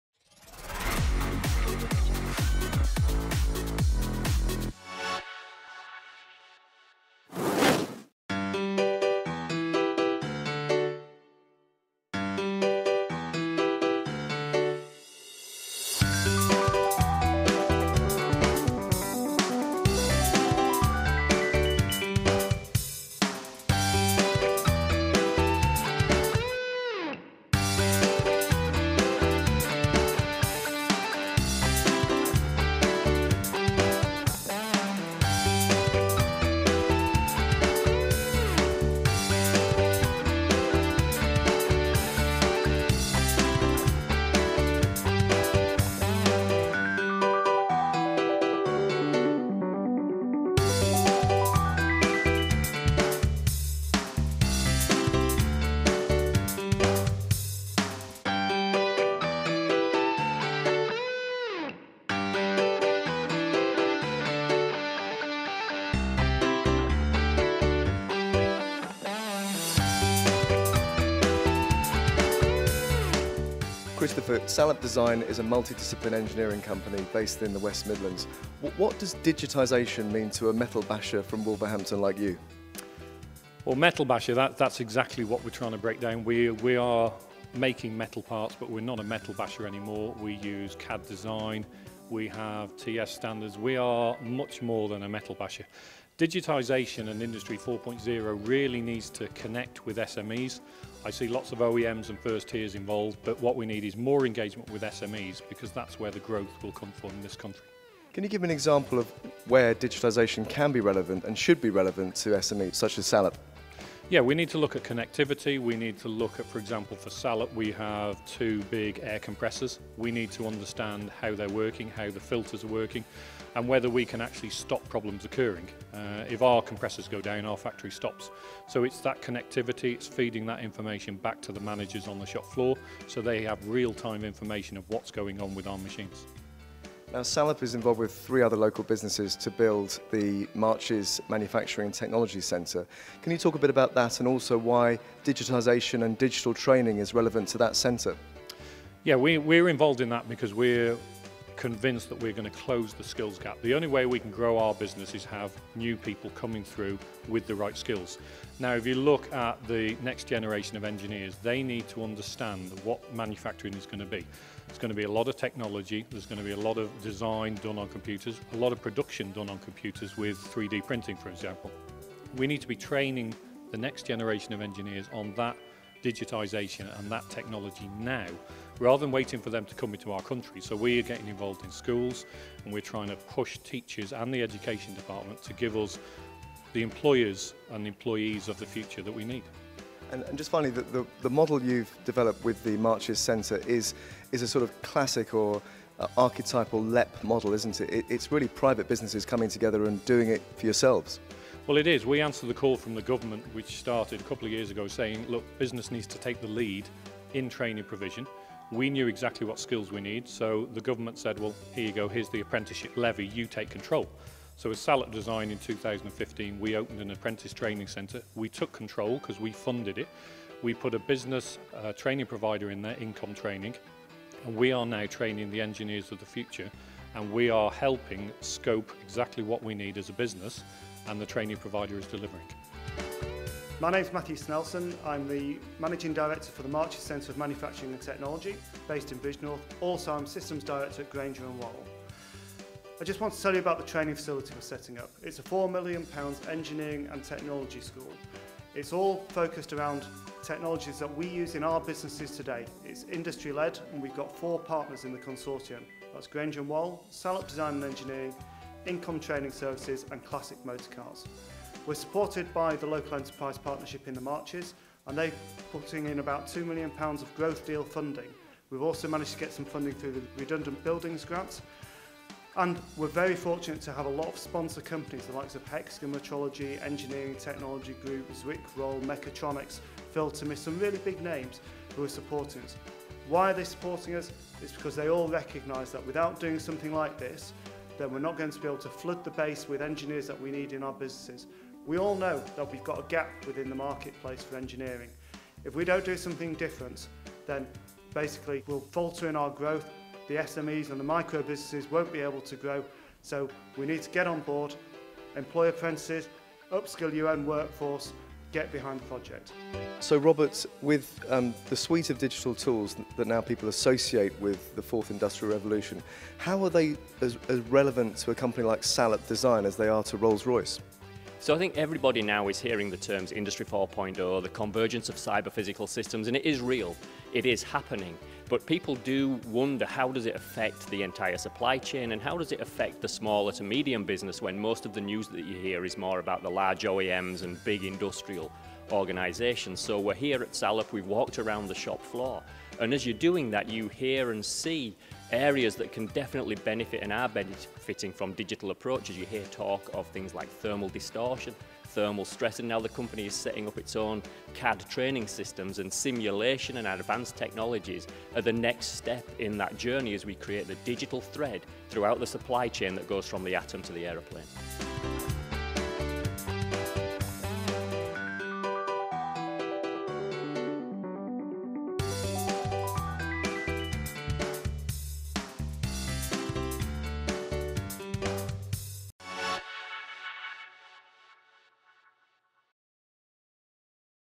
Salop Design host CATAPULT event
What does 4IR or Industry 4.0 mean to the West Midlands metal-basher?
Digital manufacturing experts from Warwick Manufacturing Group (WMG) visit Salop Design & Engineering in Shrewsbury to debate how some of the digital technology can benefit businesses like this pressings and powder coating company. Keen to avoid the “metal-basker” moniker, Commercial Director at Salop Christopher Greenough explains to 20 guests how it is modernising and using more factory floor analysis.
Salop is also part of a four-company group investing with the Marches LEP in a new training centre, the Marches Centre of Manufacturing and Technology, which will train apprentices in a mix of new digital and traditional engineering skills. WMG is part of the HVM Catapult which is running a series of tours and round tables debates about 4IR around the UK.